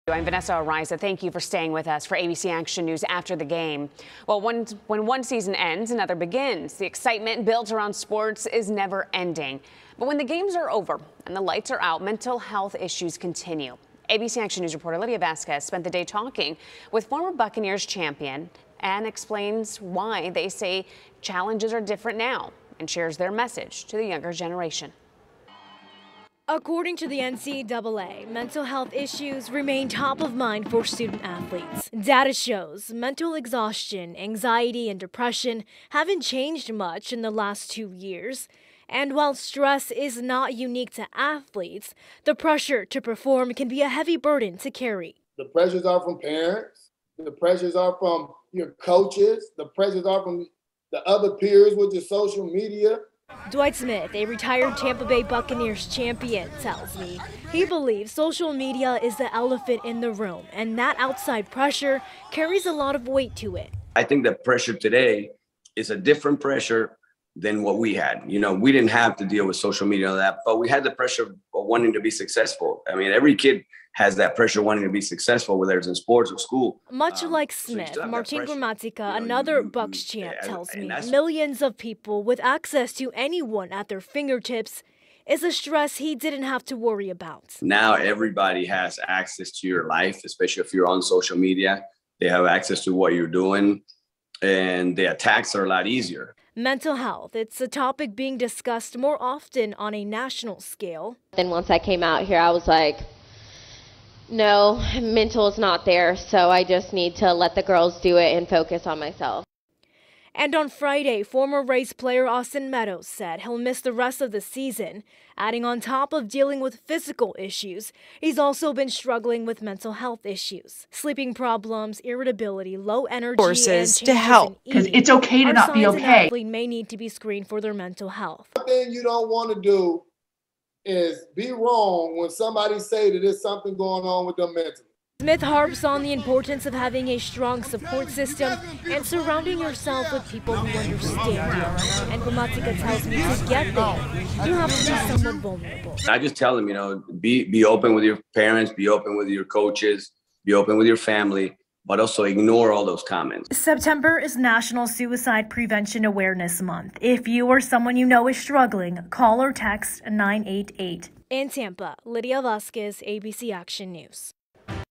Mental health and sports
Professional athletes open up about mental health struggles.